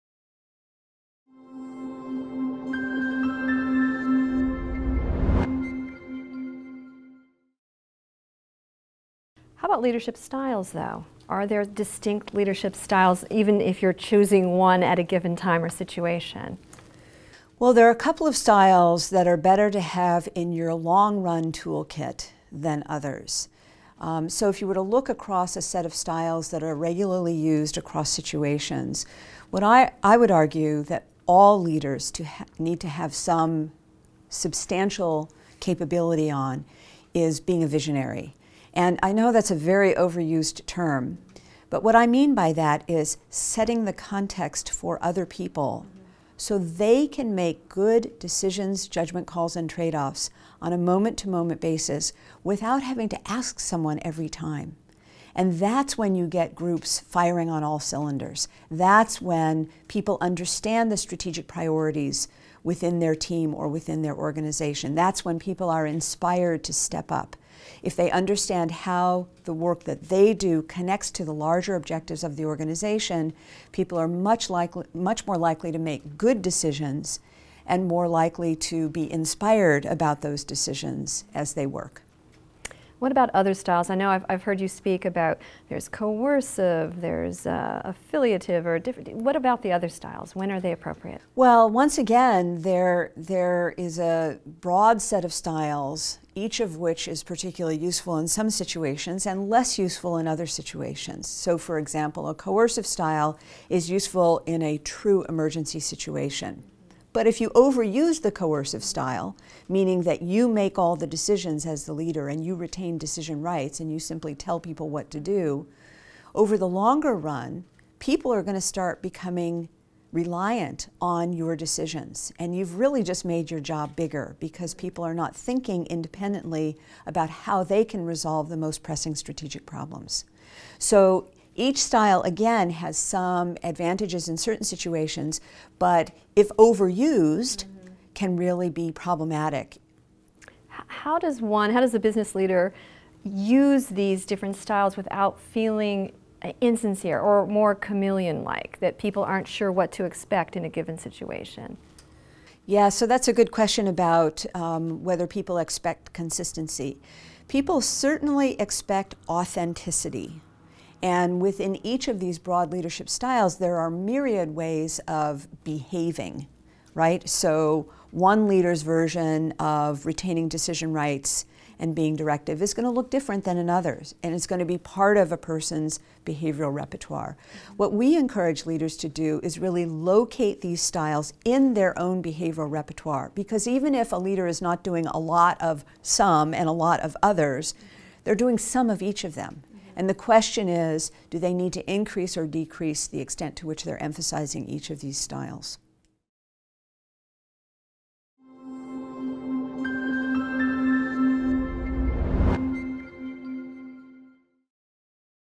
Dr. Jennifer Chatman on Leadership Styles | UC Berkeley Executive Education
Dr. Chatman is the Paul J. Cortese Distinguished Professor of Management with the Haas Management of Organizations Group. She teaches, researches, and consults on leveraging organizational culture, leading change, and managing complex teams.